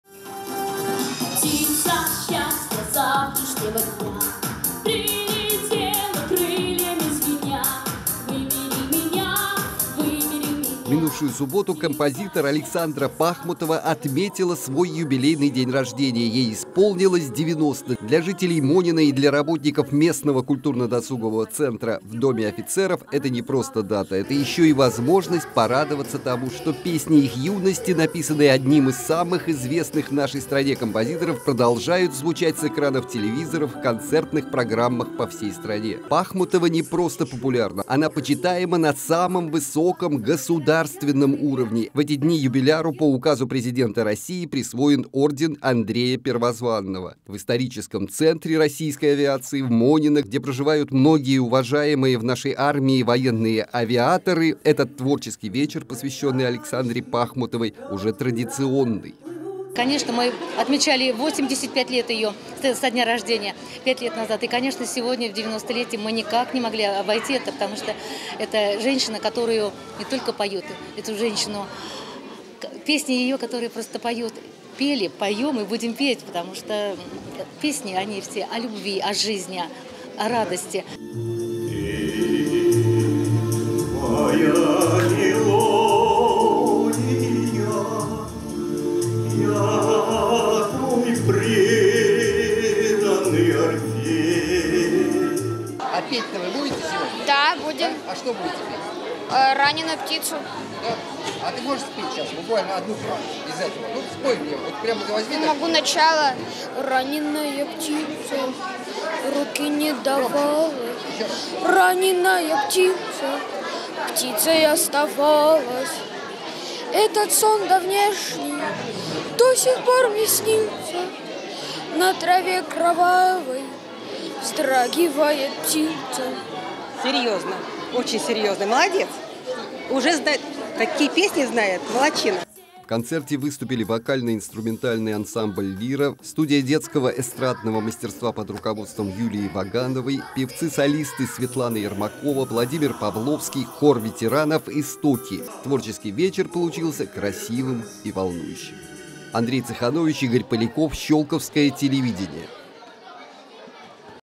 Юбилей Александры Пахмутовой
Юбилей выдающегося российского композитора Александры Пахмутовой отметили в Монино: сотрудники Культурно-досугового Центра Дома офицеров провели творческий вечер, посвященный любимому композитору. Вместе с жителями посёлка знаменитые песни автора слушал Андрей Цеханович.